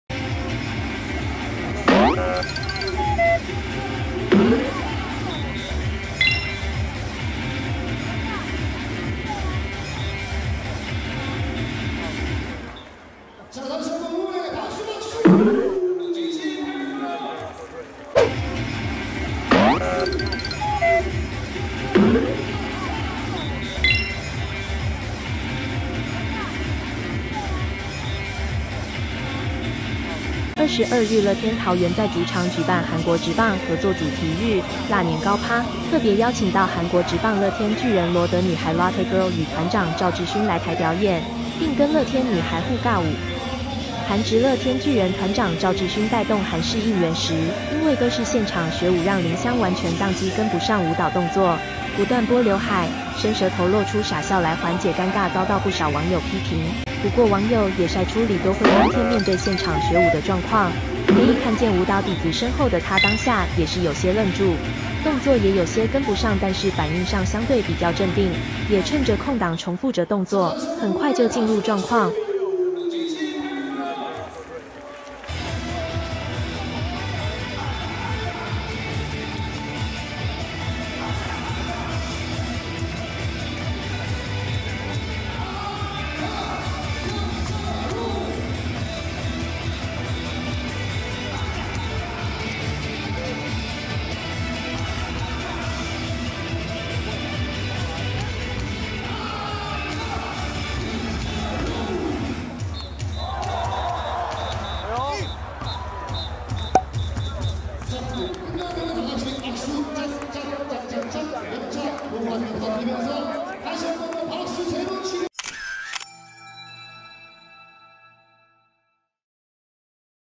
尬舞Lotte Girl！不只林襄當機 網曝：連李多慧也愣住｜三立娛樂星聞
本質球迷 提供